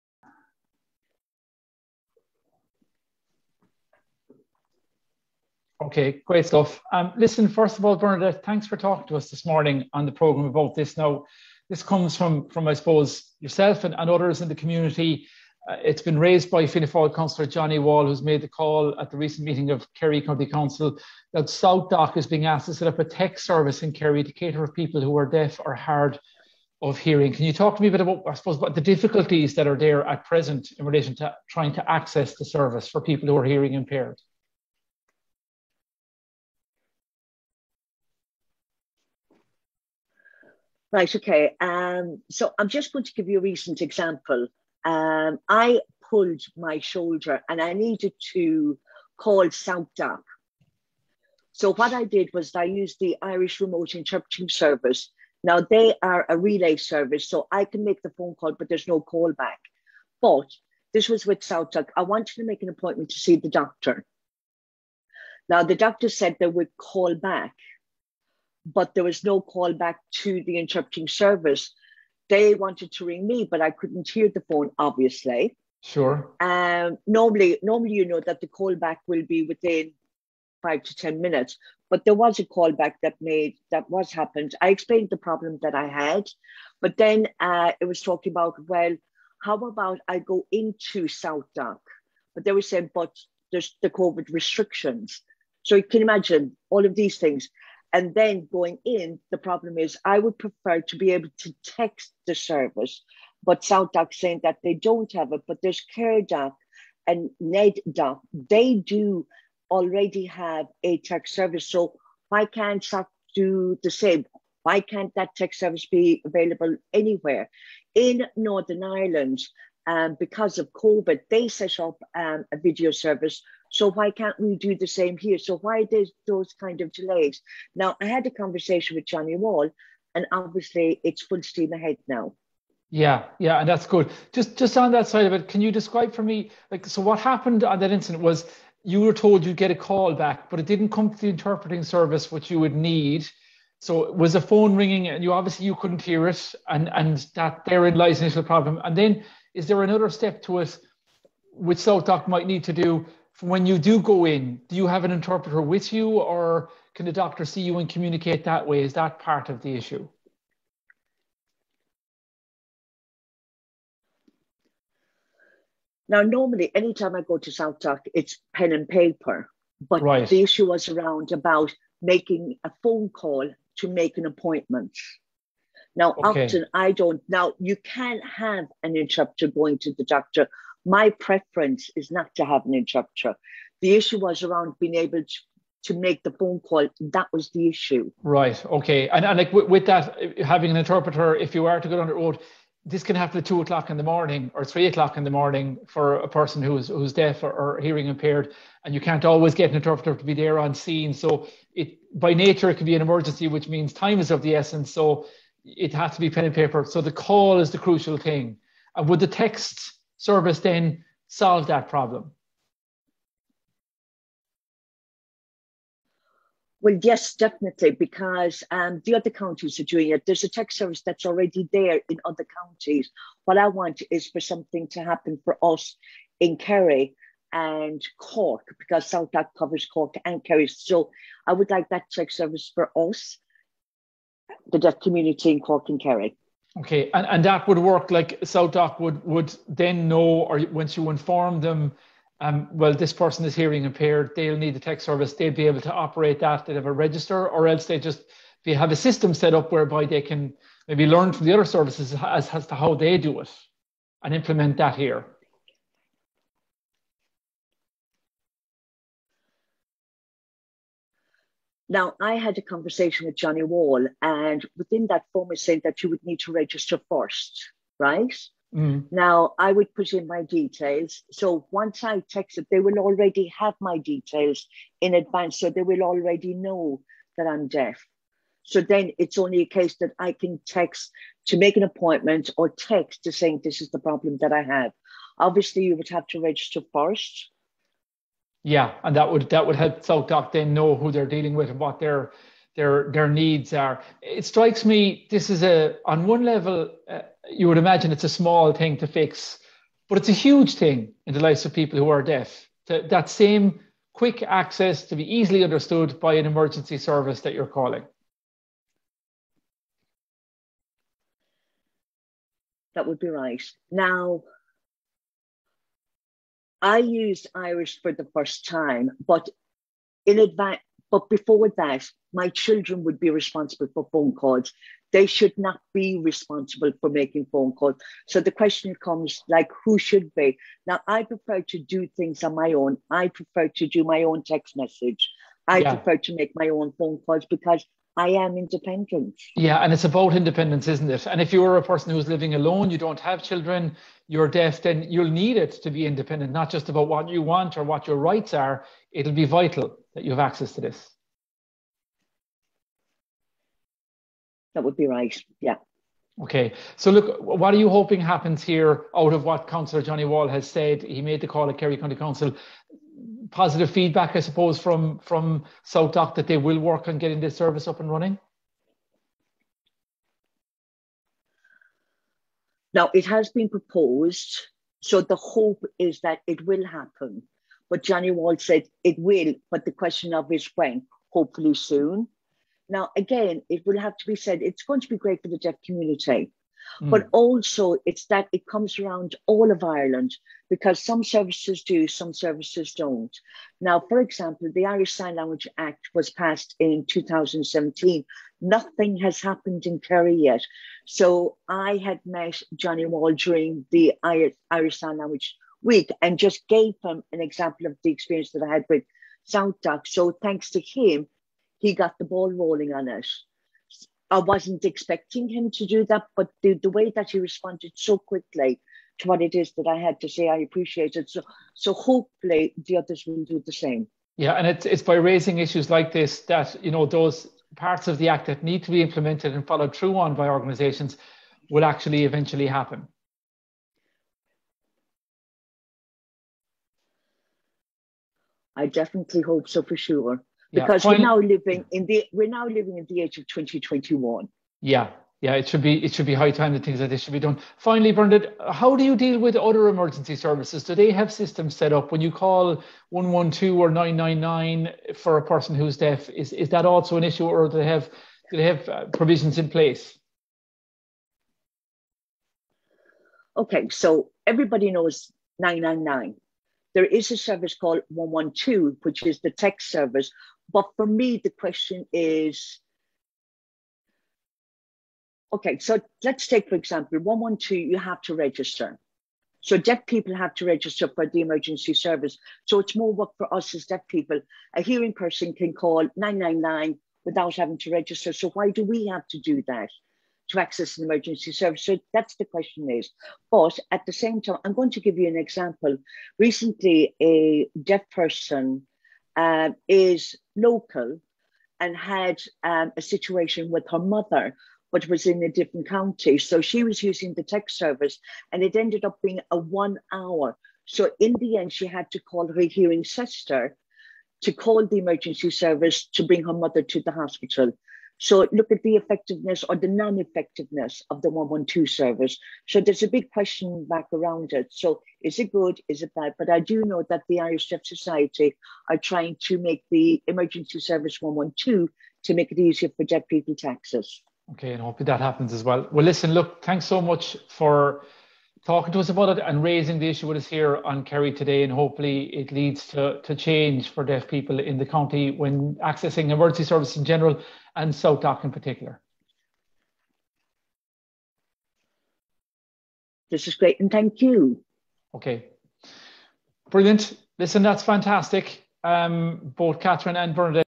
SouthDoc asked to set up text service in Kerry to cater for people who are deaf
SouthDoc is being asked to set up a text service in Kerry to cater for people who are deaf or hard of hearing. Jerry spoke to Bernadette O’Connor, who is Deaf and has had problems accessing the service.. (Bernadette spoke through interpreter Catherine White.)